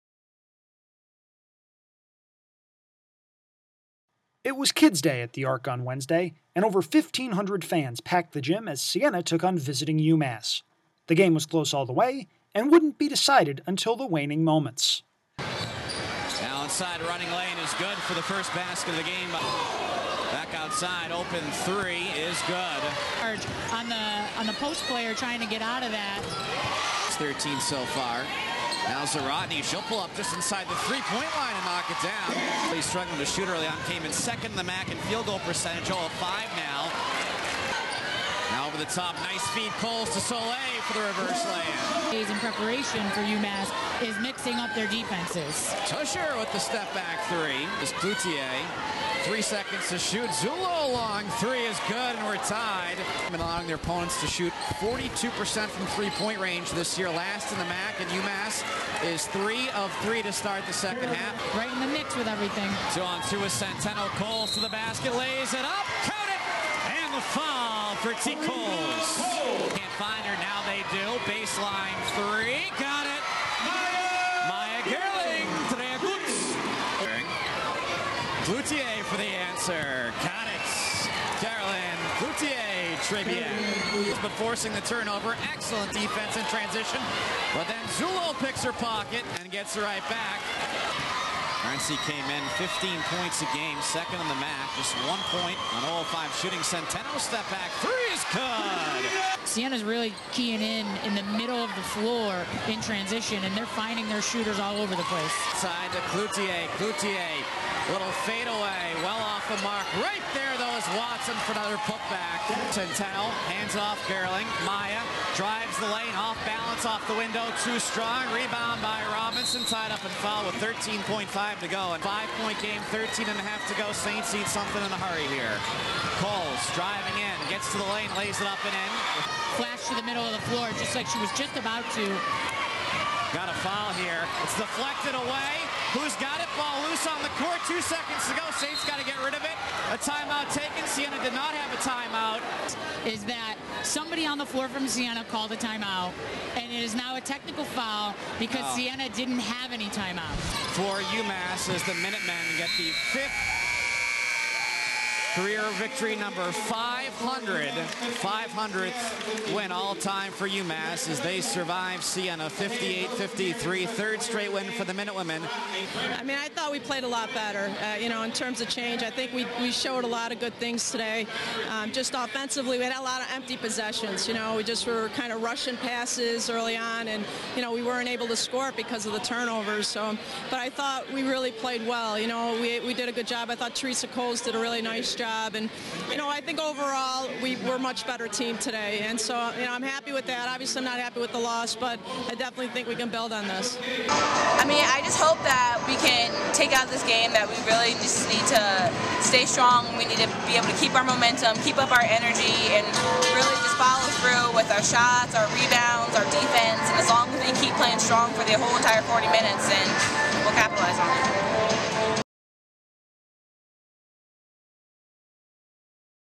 Siena-UMass Highlights
Highlights from Siena's 58-53 loss to UMass at the 2011 Blue Shield of Northeastern New York Kid's Day game.